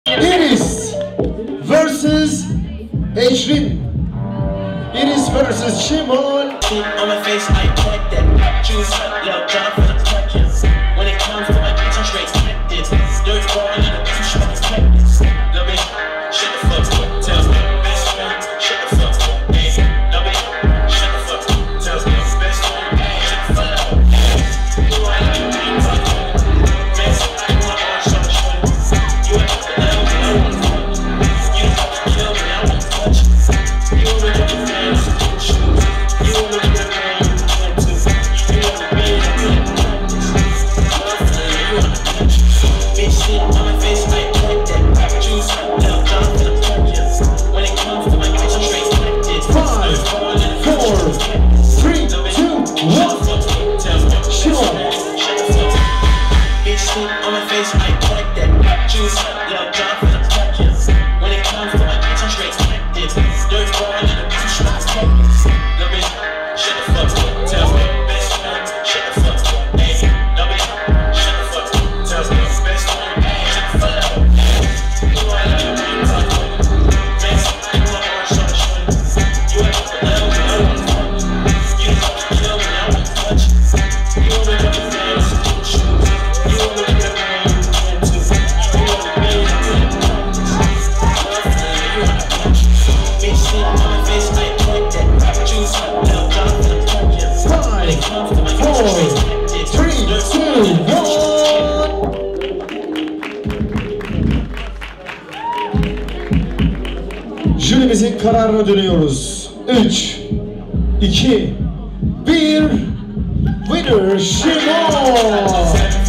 1vs1 HIPHOP KIDS Şimal ECRİN vs Twinkle SPG 2024
SPECIAL PLAYGROUND
28 Ocak 2024

JÜRİ : Stalamuerte Aydan UYSAL Bboy Jester
DJ: Volkan ARICI
HOST: By Foggy

YER:  Maze Alsancak  ​
Türkiye / Izmir

Specil Playground İzmir'de gerçekleşen, profesyonel ve amatör dansçıların bir araya geldiği hip hop dans yarışması etkinliğidir.

Bugünün en iyileri ve yarının umut vadedenleriyle gerçekleşen harika bir dans ziyafeti. Farklı şehirlerden gelen ve farklı dans stillerine sahip dansçılar yeteneğini tazelemek ve üst seviyelere taşımak için bir birleriyle yarışmaktadır. Hatırlarınızdan silinmeyecek tecrübeler yaşayacağınız dans dolu bir parti.